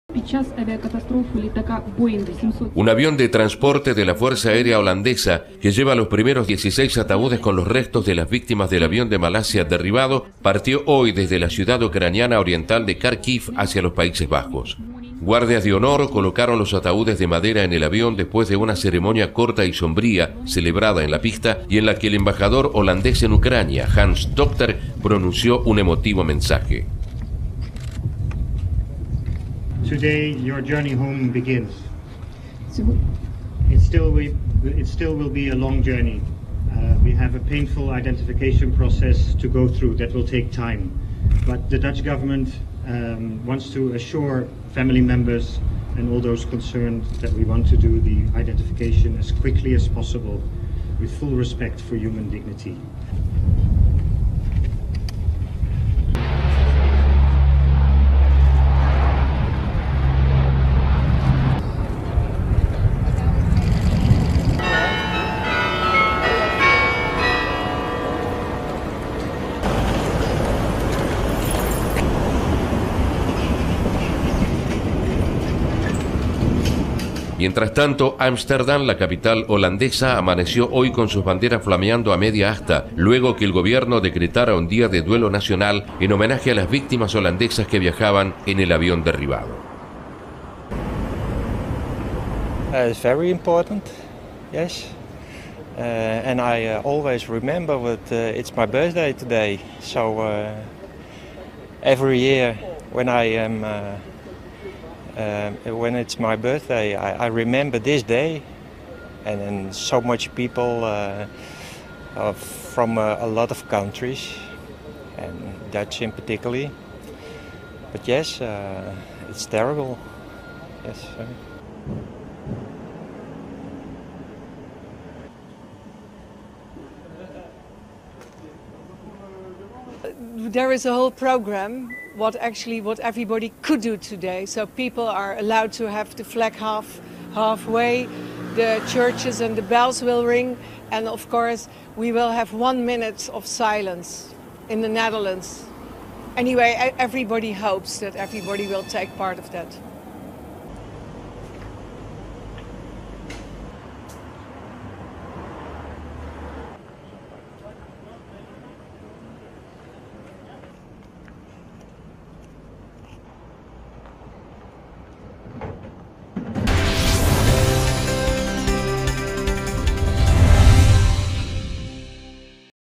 Trasladaron a Holanda los restos de las víctimas del avión malayo derribado
Un avión de transporte de la fuerza aérea holandesa que lleva los primeros 16 ataúdes con los restos de las víctimas del avión de Malasia derribado partió desde la ciudad ucraniana oriental de Kharkiv hacia los Países Bajos.

Guardias de honor colocaron ataúdes de madera en el avión después de una ceremonia corta y sombría, celebrada en la pista antes de despegar.

Las 298 personas a bordo del Boeing 777 de Malaysia Airlines perecieron cuando fue derribado el pasado jueves sobre el territorio controlado por los rebeldes en el este de Ucrania. Muchas de las víctimas eran holandeses y los Países Bajos realizarán su identificación.

"Hoy comienza su viaje a casa. Sin embargo, será un viaje largo. Tenemos un proceso de identificación doloroso que pasar por eso llevará tiempo. Pero el gobierno holandés quiere asegurar a los miembros de la familia y todos los interesados ​​que queremos hacer la identificación como más rápido posible, con pleno respeto de la dignidad humana ", dijo el representante de la embajada holandesa en Kiev, Hans Docter, durante la ceremonia.

Mientras tanto, la ciudad de Amsterdam amaneció con sus banderas a media asta dado que el gobierno decretó día de duelo nacional en homenaje a las víctimas del avión.
Los Países Bajos están tomando la iniciativa en la identificación de las 298 víctimas del accidente del jueves, 193 de los cuales eran holandeses.